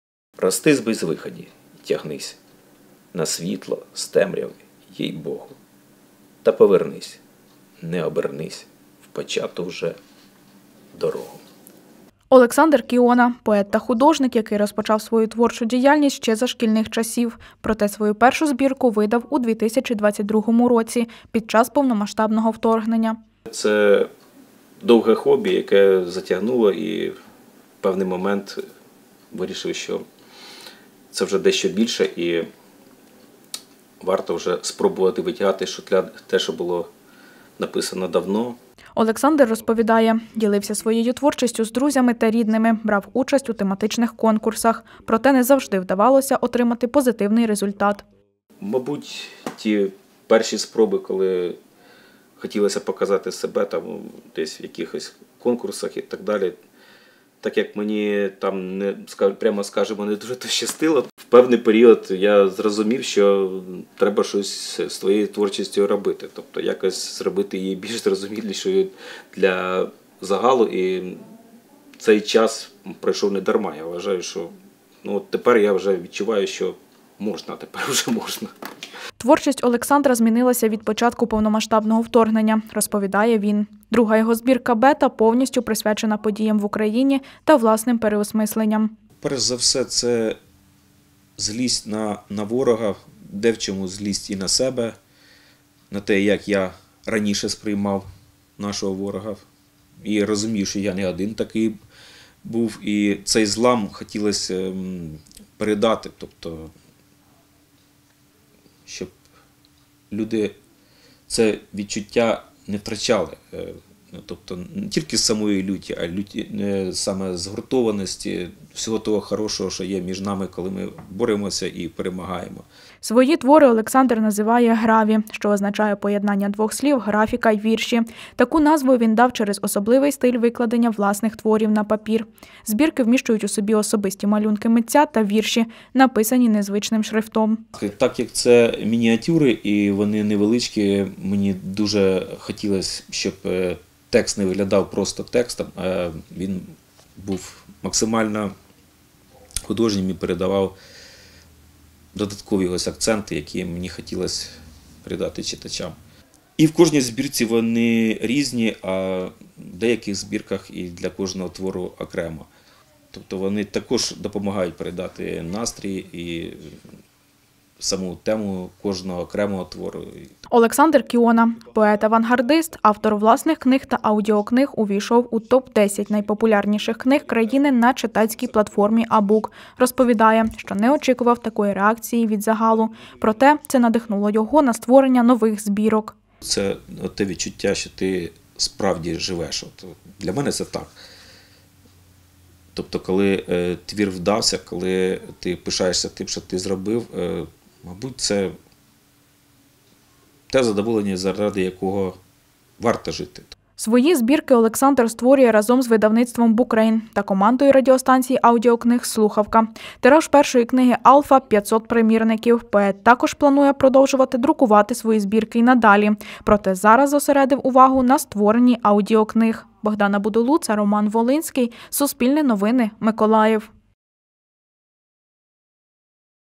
Перезавантаження творчості та рефлексія на війну: історія миколаївського поета
Олександр Кіона — поет-авангардист, автор власних книг та аудіокниг з Миколаєва увійшов у топ-10 найпопулярніших авторів країни на читацькій платформі Абук. Пише під псевдонімом Майстер Кіо. Першу свою збірку "АлФа" поет видав у 2022 році, під час повномасштабного вторгнення. З того часу поет опублікував ще три, у яких відображаються події сьогодення.